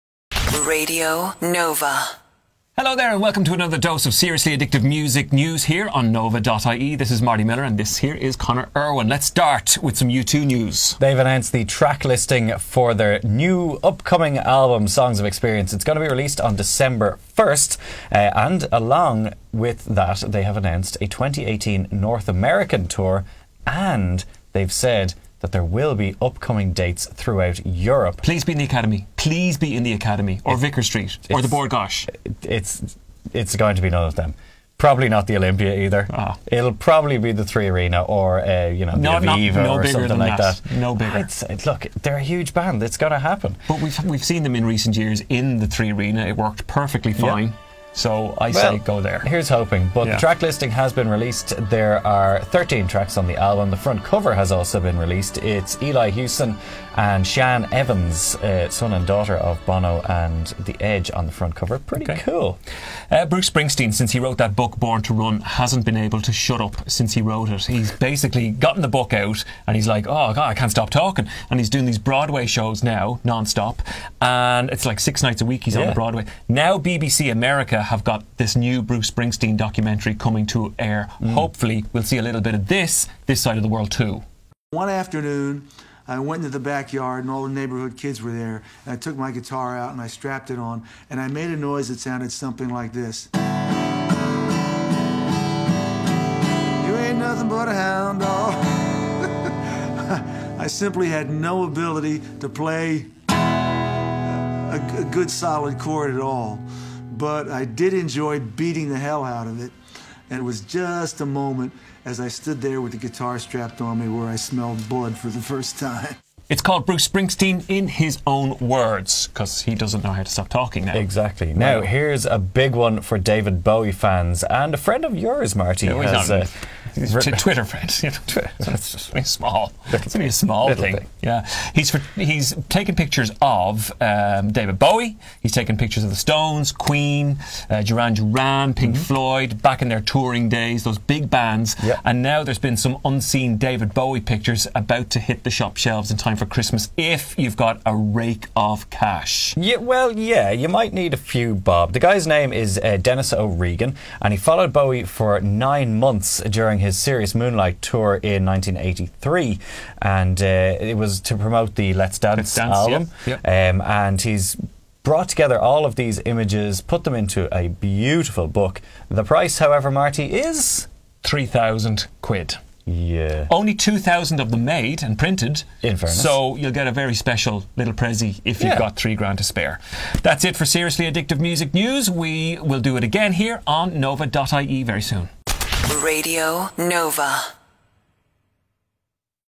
Seriously Addictive Music News with Marty & Conor - U2 & Bruce Springsteen!
Get some U2 news and a first look at a new Bruce Springsteen doco in today's Seriously Addictive Music News with Marty & Conor